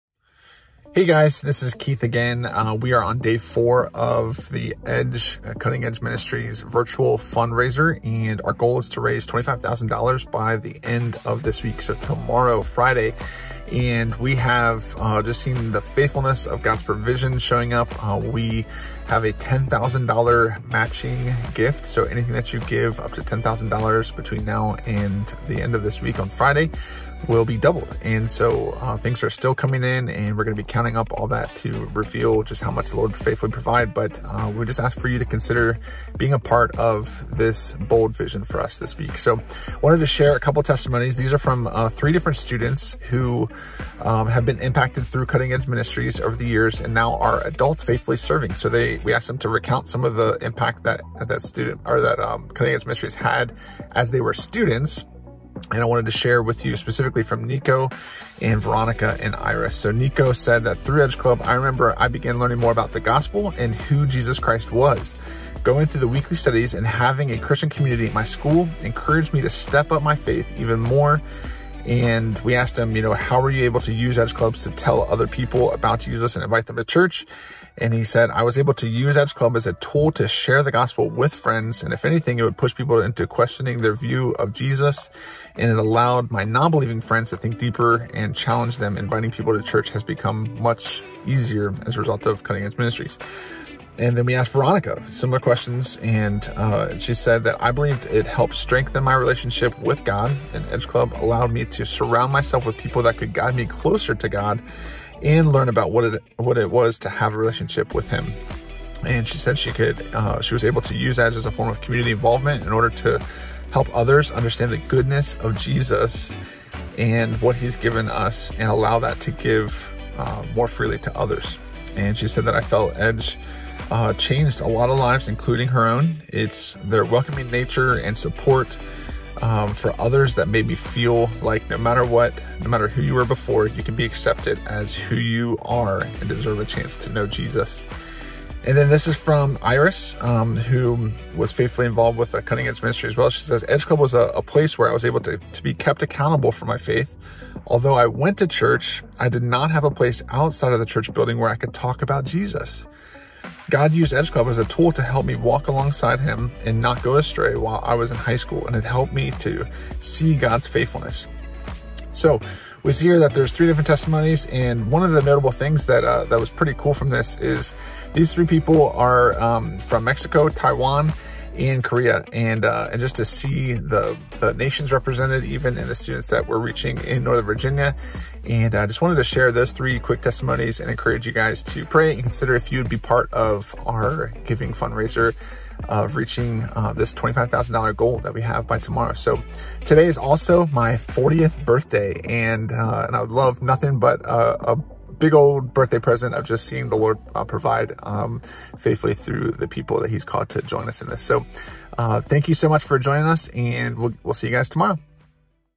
The Global Impact of Cutting EDGE Ministries: Changing Nations One Student at a Time
If you missed yesterday's video update, we are praying for the Lord to provide $25,000 this week to help us scale our youth leader network to 1,000 church partnerships and train 10,000 student missionaries in the next year.
Would you consider giving today to make this vision a reality?
All gifts up to $10,000 by Friday will be doubled by a generous matching donor.